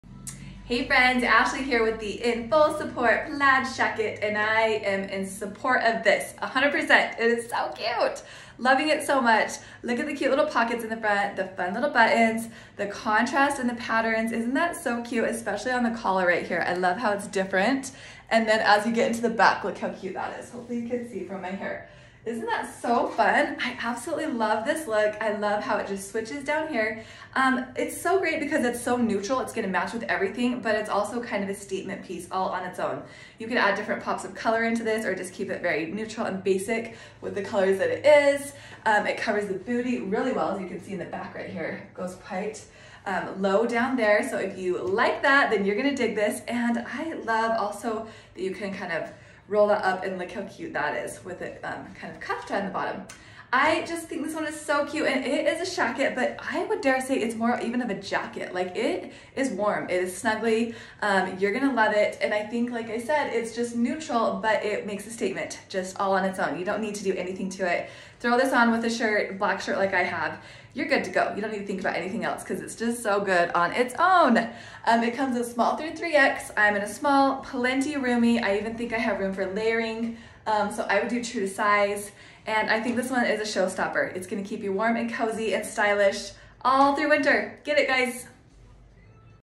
In Full Support Plaid Shacket
We are in full support of you getting this shacket! It's trendy and fun and PERFECT for the season! Featuring a contrast detail, collared neckline, long sleeves, plaid print, and button-down front closure, this shacket is just what you'll need!

Get the look: Pair the In Full Support Plaid Shacket with the Never Leave Paperbag Mom Jeans and sneakers!

Details

Long Sleeves
Contrast Detail
Collared Neckline
Plaid Print
Button Down Front Closure
Vendor
Davi and Dani

Fabric
100% Polyester, Contrast: 100% Polyester

Size
S-3X

Fit
True to size, relaxed

Approximate Measurements
Small: Bust 46", Length 27"

Medium: Bust 48", Length 28"

Large: Bust 50", Length 29"

1X: Bust 54", Length 30"

2X: Bust 56", Length 31"

3X: Bust 58", Length 32"

Model McKenna: 5'9 size 4 wearing size Small

Model Kendall: 5'5 size 14/15 wearing size 1X

Shipping
All in stock items ship within 5-7 days